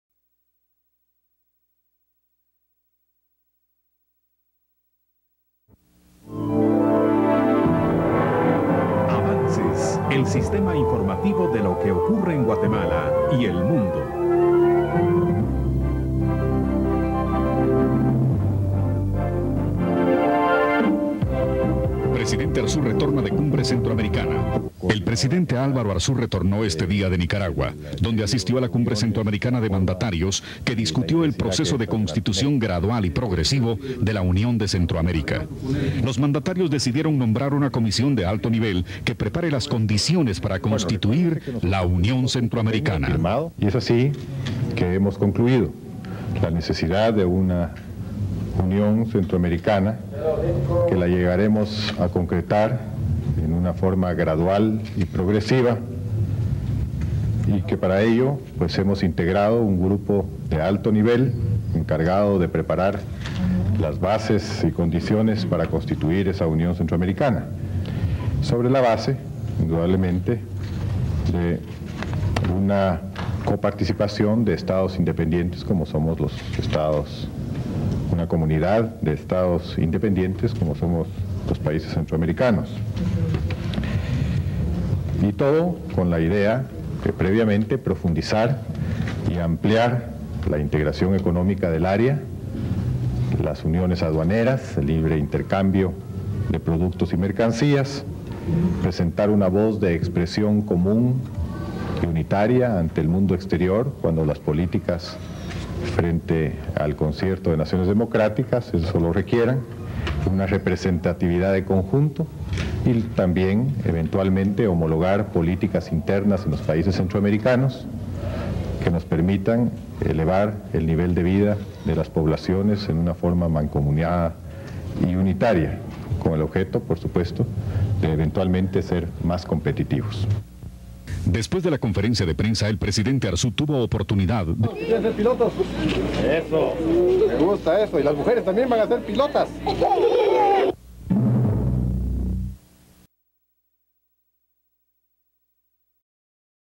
Septiembre 1997 (3 de 12)
1. Presidente Álvaro Arzú retorna de cumbre centroamericana de mandatarios.